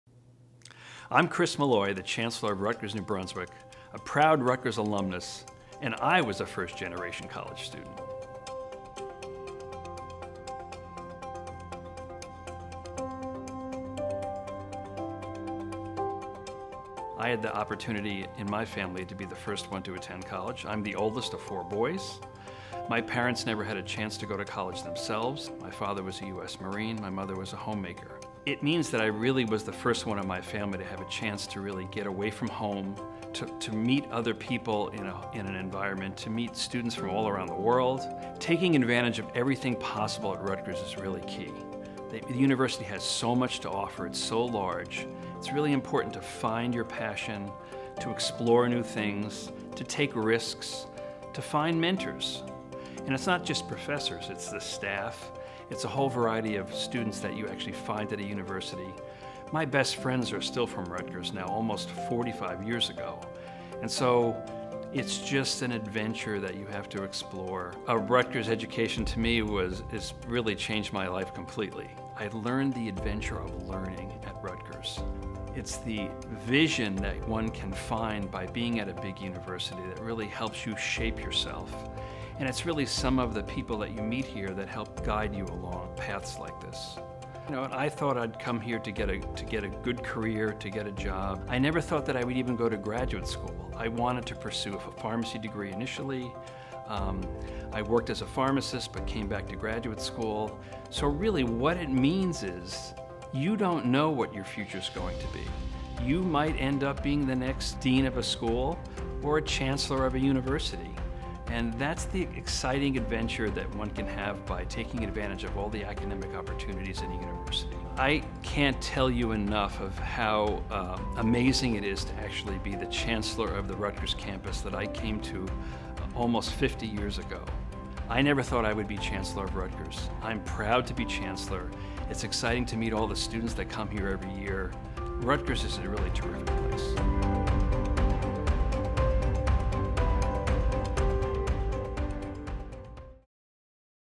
First-Generation Students
This week, Rutgers-New Brunswick will join our nation’s colleges and universities as we celebrate the contributions and achievements of first-generation students. This event coincides with the anniversary of the signing of the Higher Education Act of 1965, which opened the doors to college for millions of smart, low- and middle-income Americans by establishing need-based grants, work-study opportunities, and federal student loans.